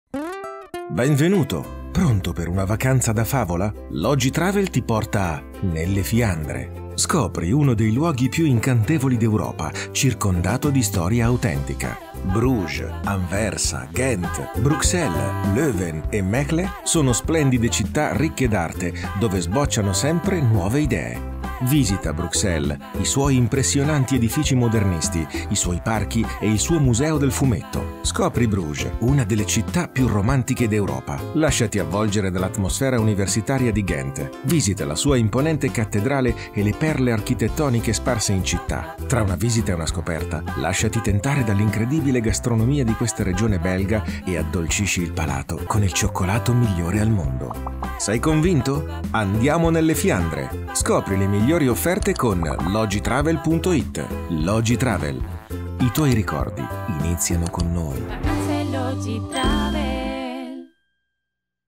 Fiandre 2018
Scopri le Fiandre dove l’arte di Rubens, la deliziosa gastronomia, l’architettura innovativa e i romantici canali creano inconsuete e inaspettate combinazioni.